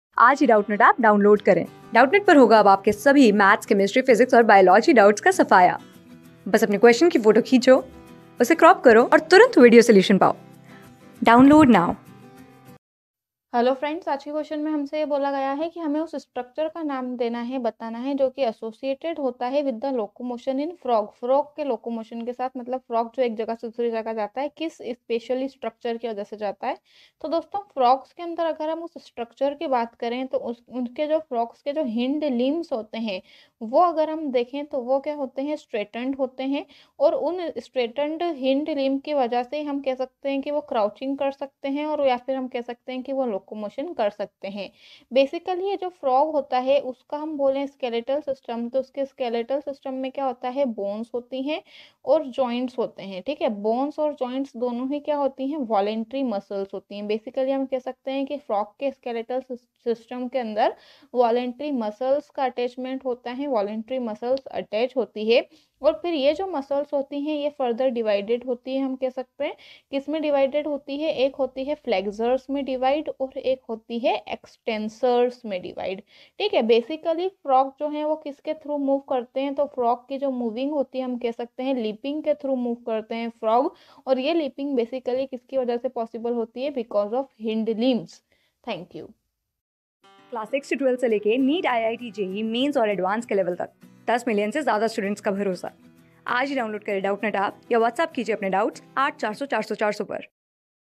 Name the structure associated with locmotion in the following organissm Frog | CLASS 7 | MOVEME...
Name the structure associated with locmotion in the following organissm Frog
Class: 7
Subject: BIOLOGY
Chapter: MOVEMENT  IN ANIMALS AND PLANTS
Board:FOUNDATION

👉 Have Any Query? Ask Us.